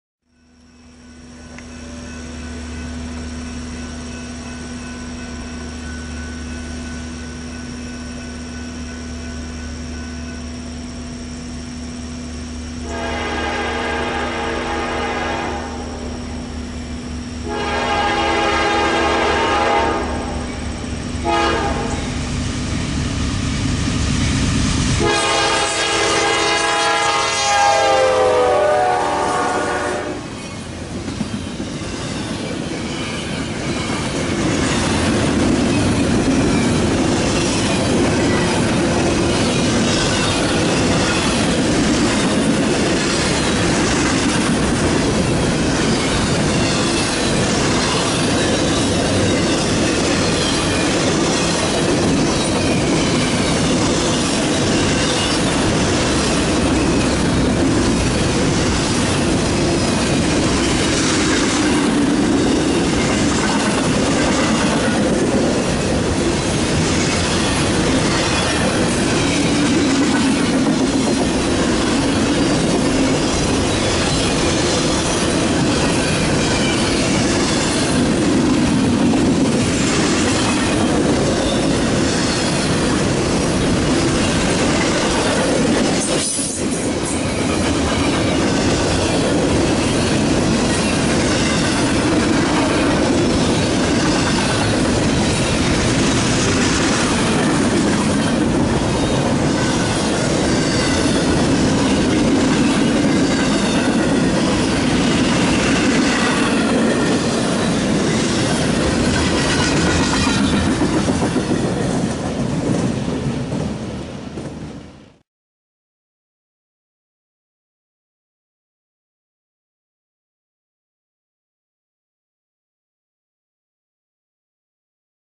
NS Intermodal 22K - NS C44-9W #9181 Beautiful K5LA Doppler Effect & AC44C6M #4180 - Madison OH
Here is video & pictures from Madison Ohio in August 2019 of fast 50MPH NS Norfolk Southern eastbound intermodal train 22K with 89 wells of containers all single stacks + a few baretables on the rear, lead by 2 GE's General Electric's NS C44-9W Dash 9 #9181 with a nice sounding Nathan 5 chime K5LA / K5LAR24 train horn doppler effect & NS AC44C6M #4180 trailing 2nd.

Filmed: Tuesday August 13th, 2019.

Corner Field Hobby Railfan.